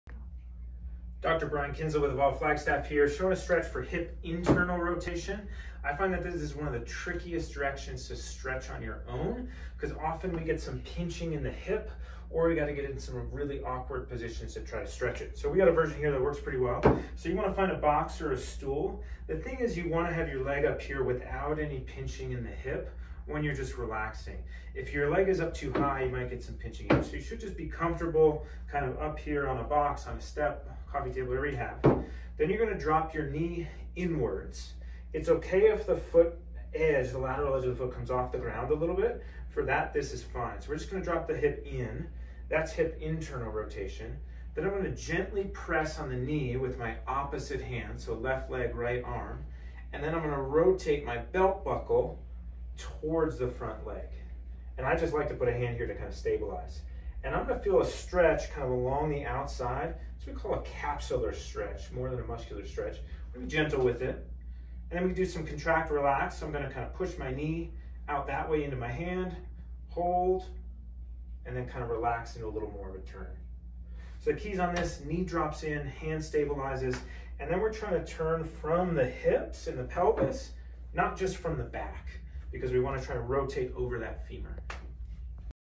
Hip Internal Rotation Stretch
Internal rotation can be a tricky direction to stretch, but this can be a more simple way to get after it.